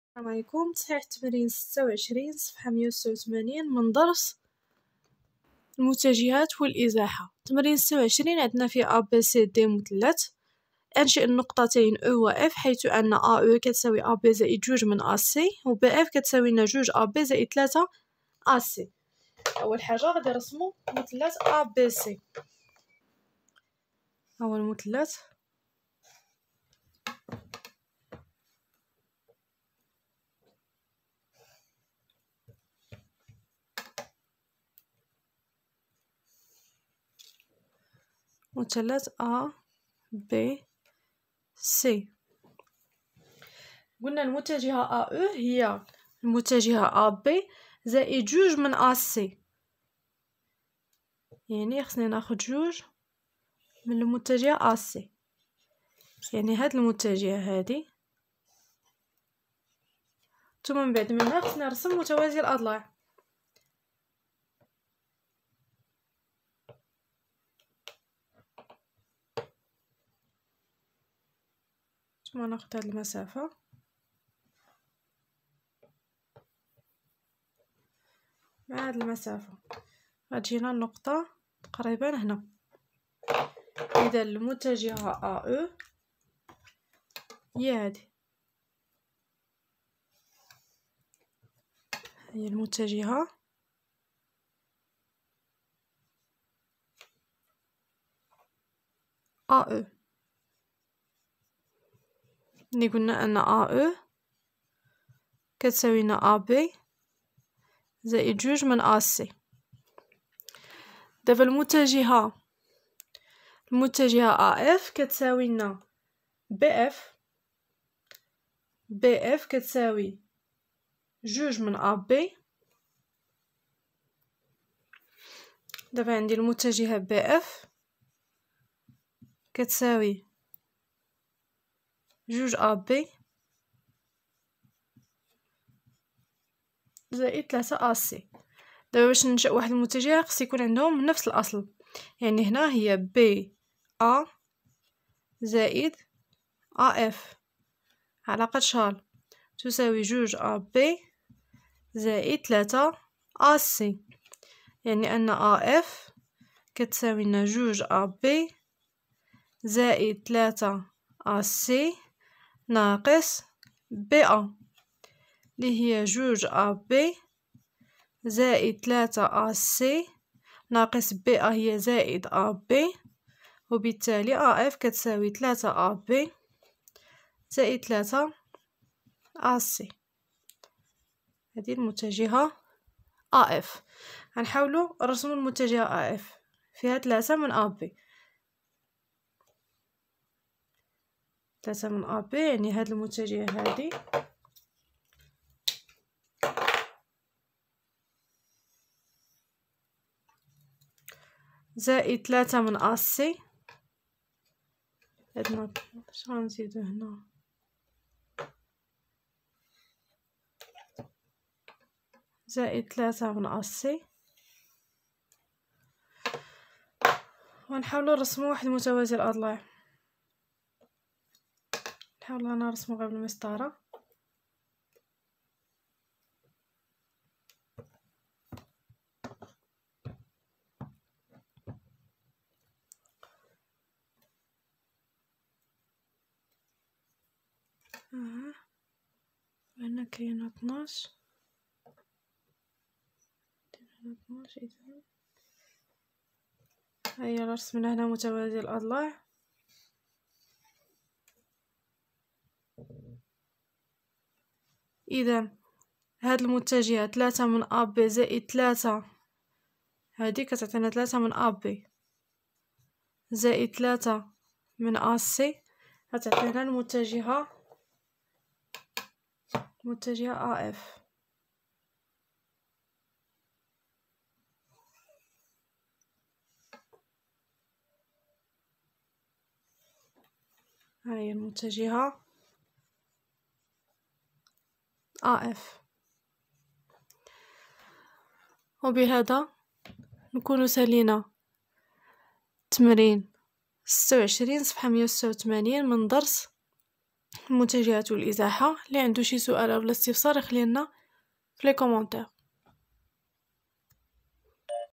حل التمرين 26 الصفحة 186 كتاب المسار في الرياضيات للسنة الثانية إعدادي درس المتجهات و الإزاحة
حل التمرين 26 الصفحة 186
كتاب المسار في الرياضيات
 للسنة الثانية إعدادي
 درس المتجهات و الإزاحة